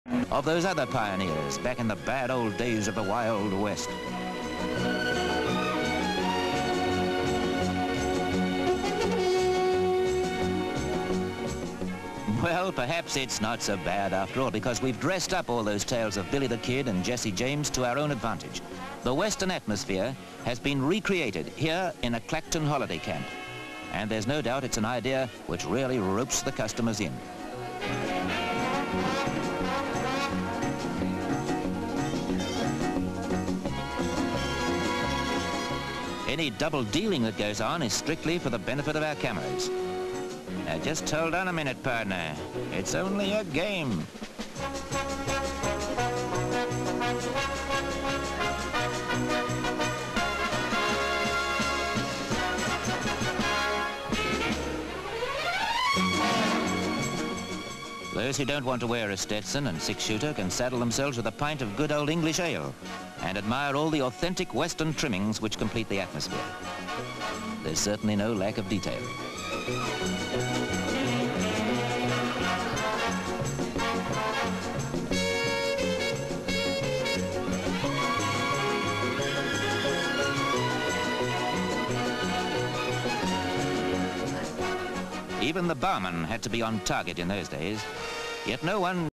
Wild West Bar (1966)
Clacton-on-Sea, Essex.

A holiday camp bar with a Wild West theme - an American saloon where you can still get pint of bitter!

M/S of a stagecoach riding past a Wild West saloon.  Point of view shot from the top of a stage coach as it rides through what looks like a Western film set.  C/U on sign for "Western Saloon".  M/S of a cowboy leaning on a post on the veranda of the saloon.  M/S of two cowboys coming through the swing doors of the saloon.  Various C/Us of Wild West artefacts hanging on the saloon wall.

M/S of a man in a sombrero swinging a lasso.  C/U of the lasso landing on a horse head hanging on the wall.  Various shots of the cowboys playing poker around a table with several pints of beer, one of the cowboys places a pretend gun on the table.  One of the cowboys loses, throws his cards on the table and exits, several holidaymakers watch from a balcony.

M/S of a family of holiday makers in modern dress sitting around a table in the saloon, a waiter serves them drinks.  The cowboys saunter over to the bar to chat up a brassy blonde in a horrible green dress.  The barman slides two pints of bitter down the bar to the cowboys.

Cuts exist - please see separate record.
 FILM ID:353.03